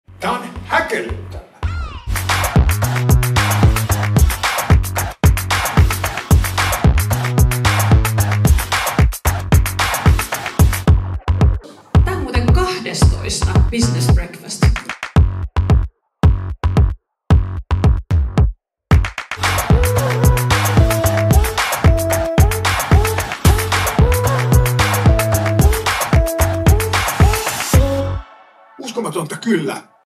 Business Breakfast promovideo 2024 by lennartmedia.fi
Business Breakfastit ovat Salossa toteutettavia verkostoitumistilaisuuksia, joissa on aina kiinnostavia kutsuvieraita mukana. Tilaisuudet on suunnattu yritysten edustajille ja muille vaikuttajille.
Järjestelyt yhteistyössä Yrityssalo, Salon Kauppakamariosasto ja Salo IoT Campus
Musiikki: GDKML8Y85KSRUHQA